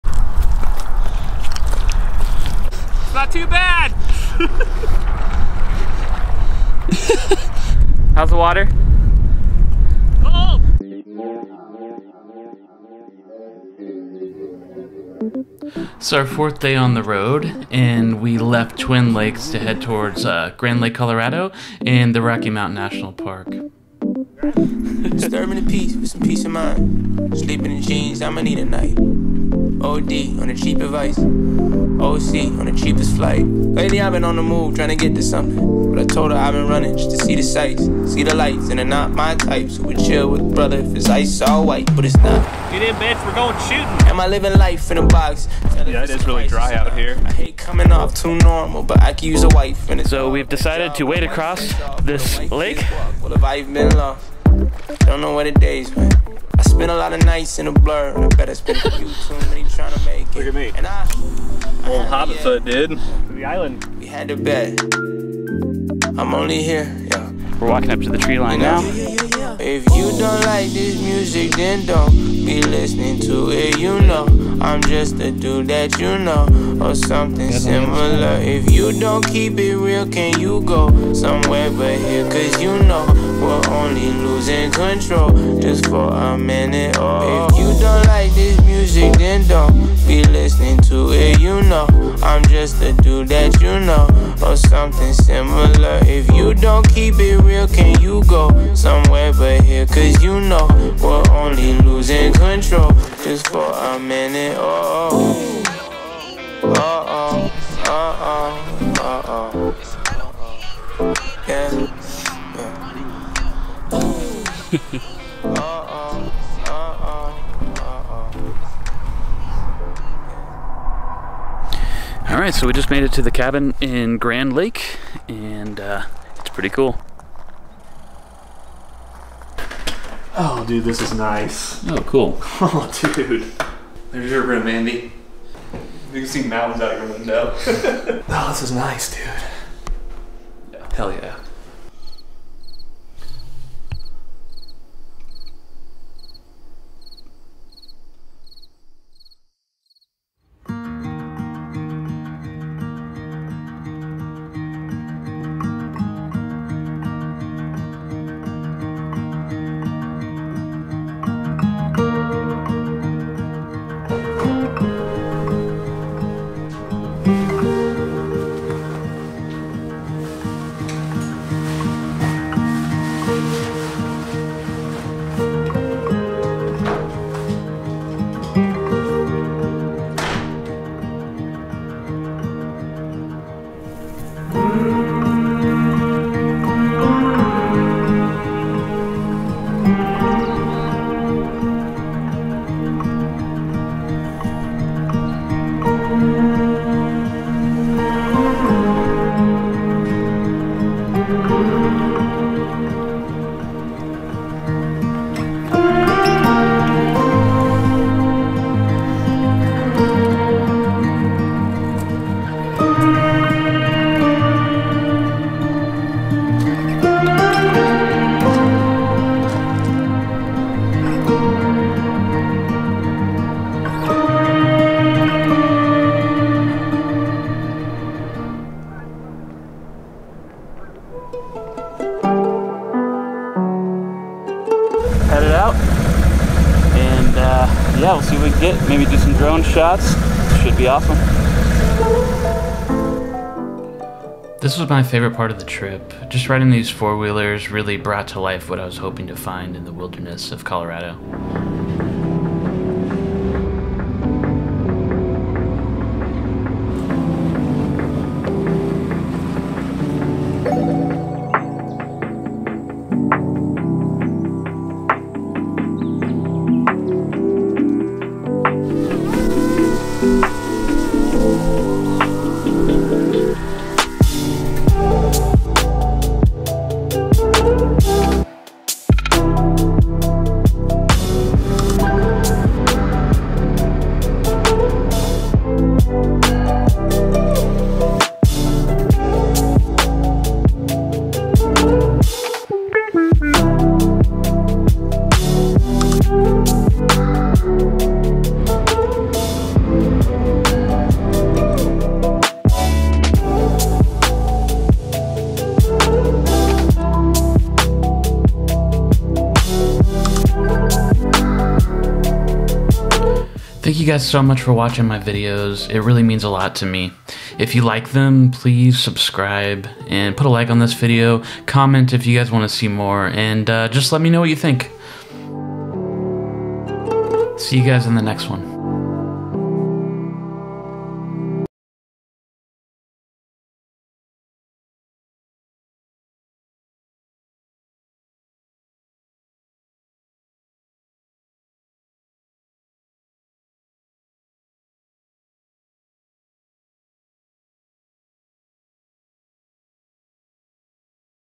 Grand Lake Memories | Colorado Road Trip (2/3)
After three days in Twin Lakes filming our friends wedding, Caleb and I set out on the road to Grand Lake and Rocky Mountain National Park. It was a great time on the road with amazing sights, and some funny detours along the way.

SOCIAL

MY GEAR

LENSES